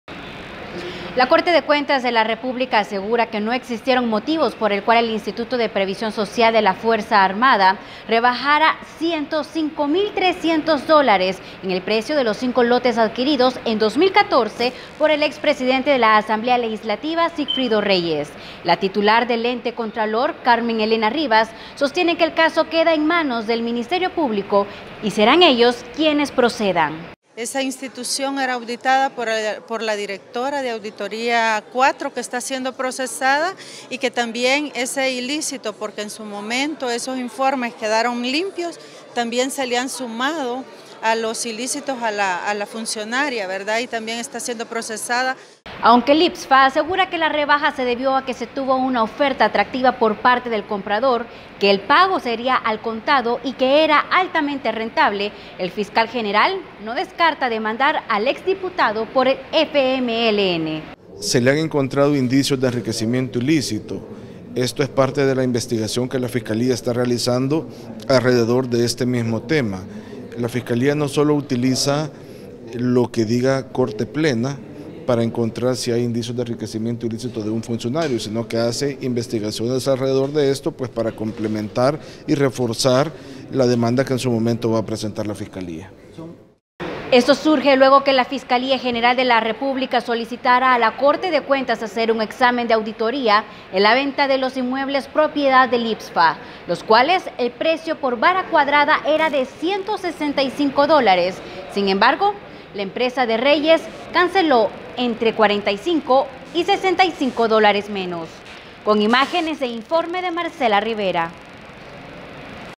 Investigan a exdiputado Sigfrido Reyes por enriquecimiento ilícito
Investigan a exdiputado Sigfrigo Reyes por enriquecimiento ilícito